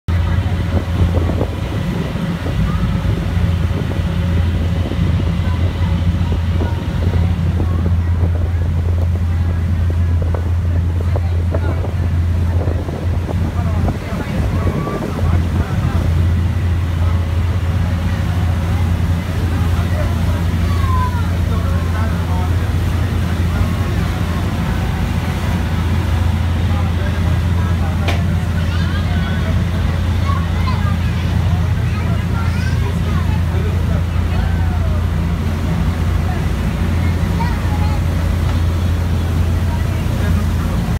Ferry ride#around Atlantis Hotel#Dubai#
Wonderful Boat ride near Atlantis Hotel in Dubai !